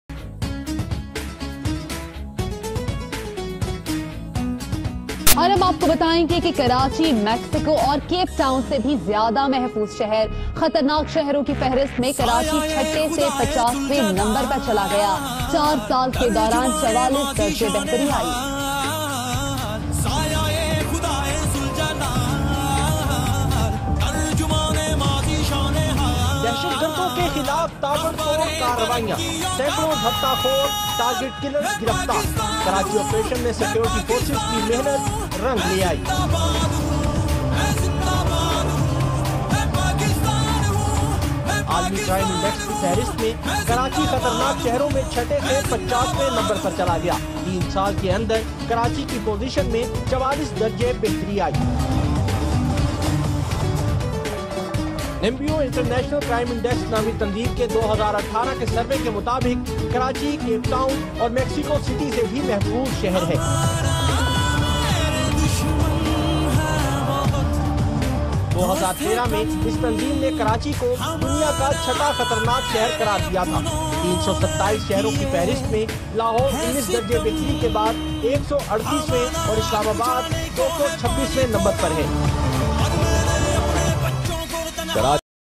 KARACHI KARACHI HA
After Few Years (Alhumduellah)Pakistan Most Impotent Tread Hub
 Karachi Is Continue Industrial  Zone And Most Top 10 Cities In The World